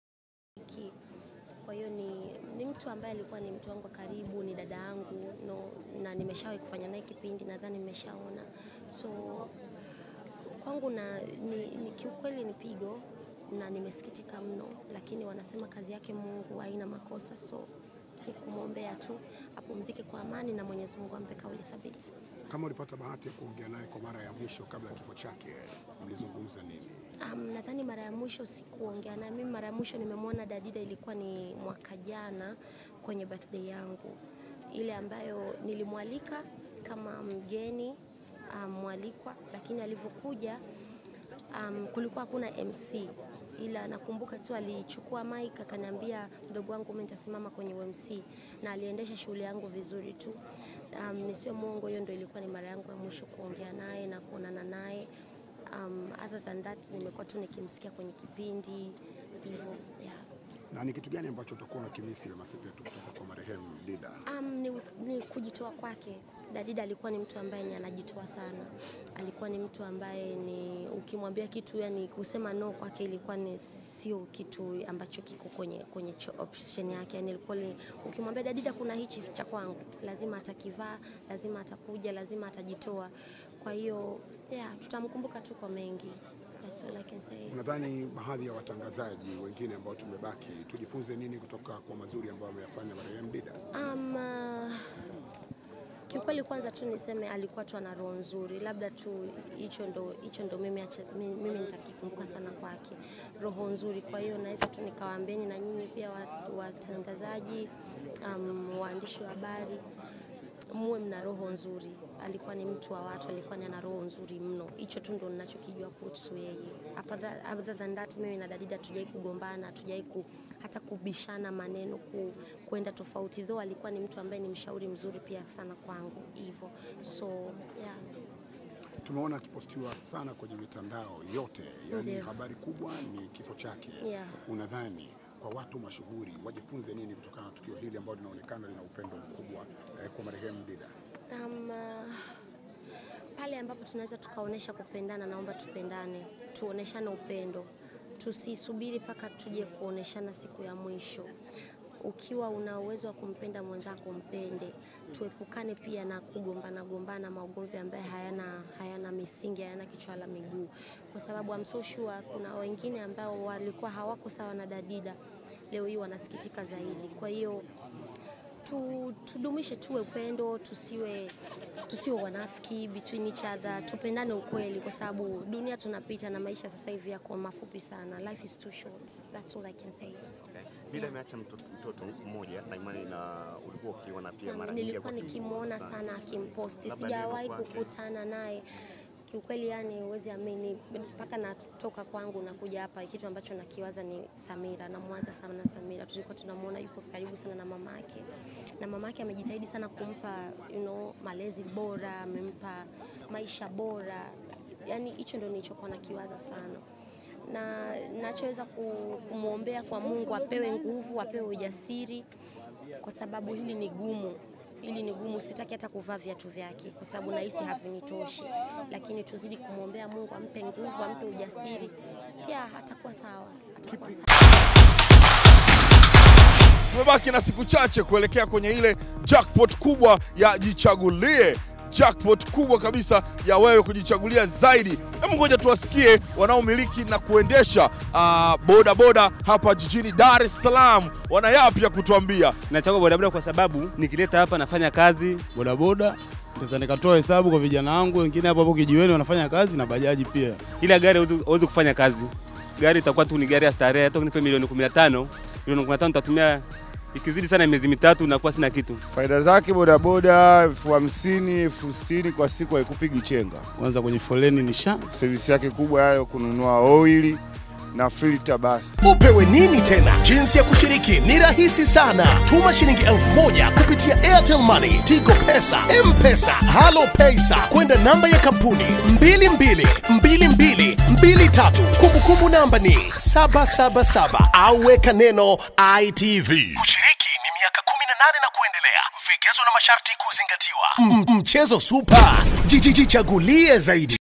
WEMA SEPETU ASIMULIA ALIVYOKUTANA NA DIDA MARA YA MWISHO 'TUPENDANE, TUACHE UNAFKI'
Muigizaji wa filamu Bongo, Wema Sepetu, amesimulia jinsi marehemu Khadija Shaibu 'Dida' alivyokutana naye mara ya mwisho siku ya bethidei yake na kuwa MC wake.